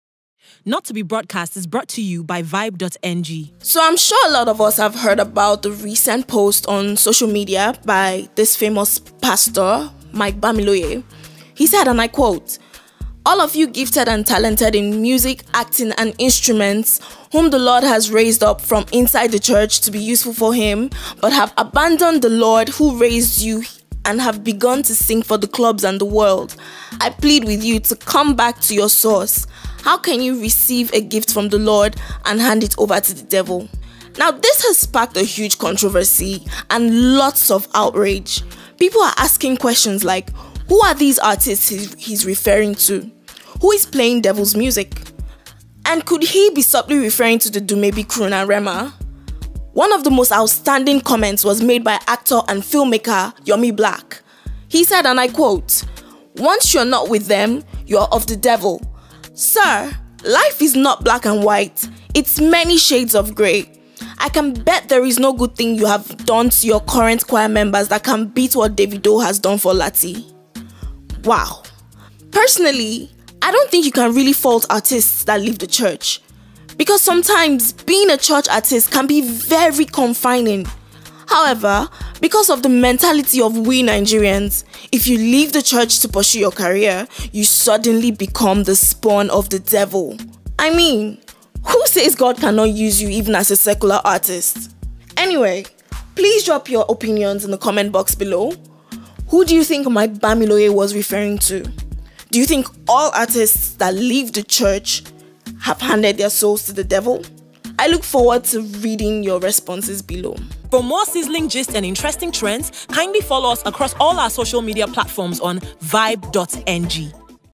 Did Mike Bamiloye Just Say Music Artistes Who Left The Church, Now Sing For The Devil? [NTBB]
On this episode of NTBB, We see Mike Bamiloye calling out former church artistes and says they are now using their talents for the devil.

There is a whole lot more to the gist. Watch this episode of NTBB to find out.